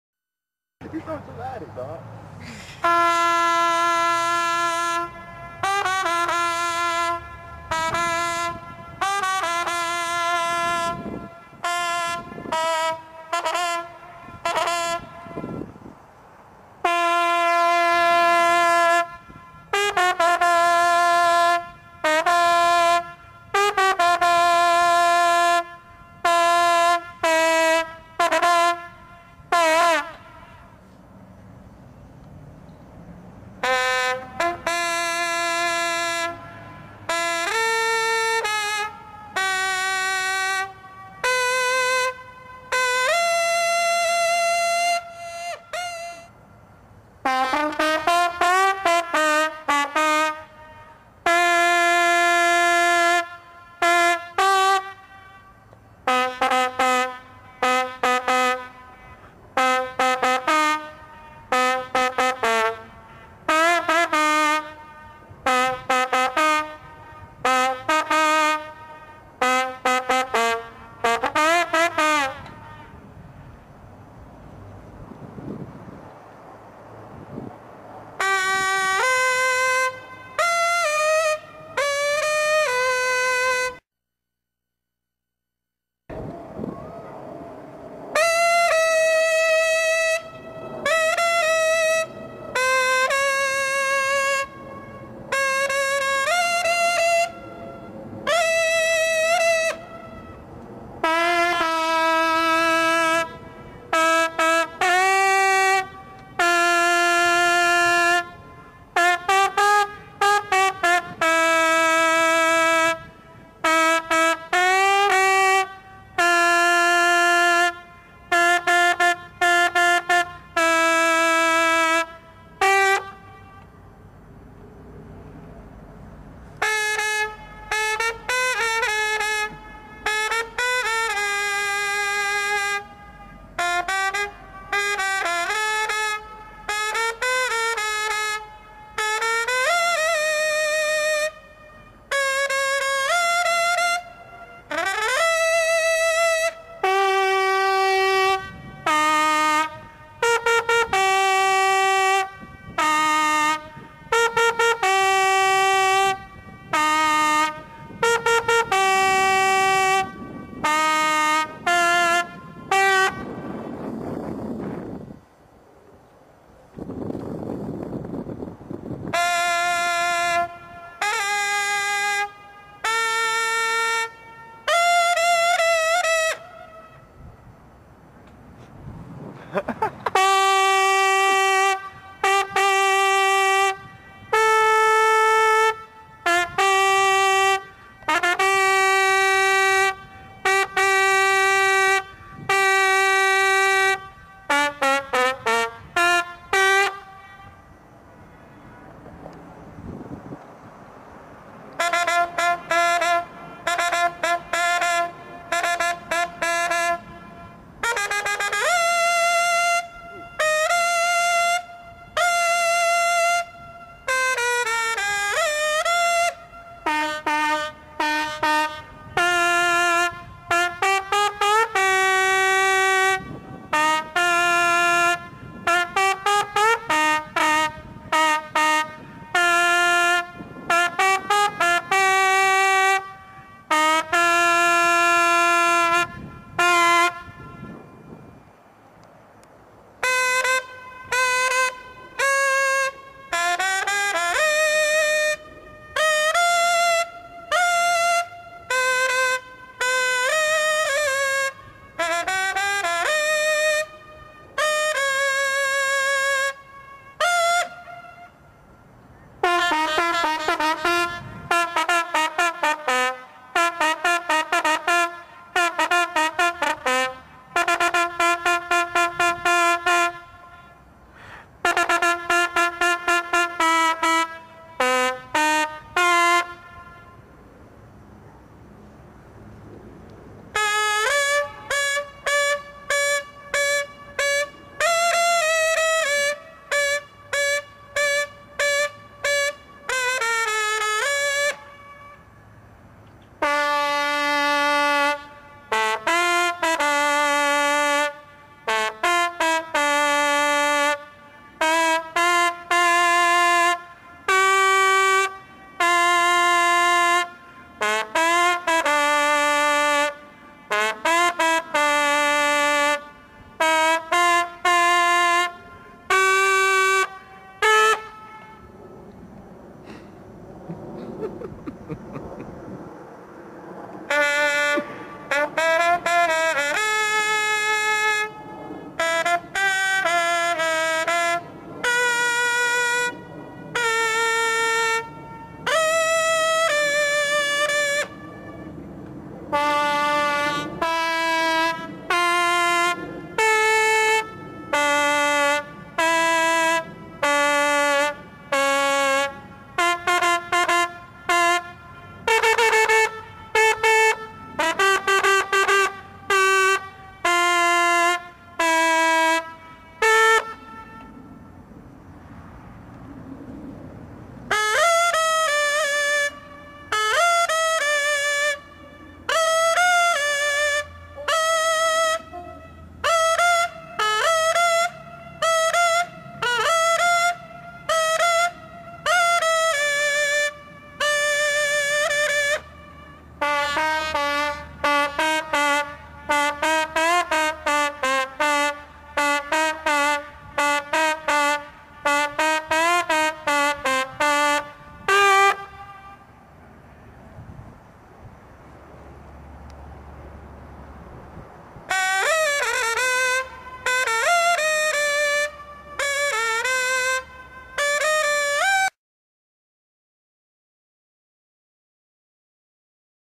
Crank Session Vol. 1 - Q vs Wok - 2007
My homies challenging in 30 degree weather (cold as hell) "KROWNS UP!"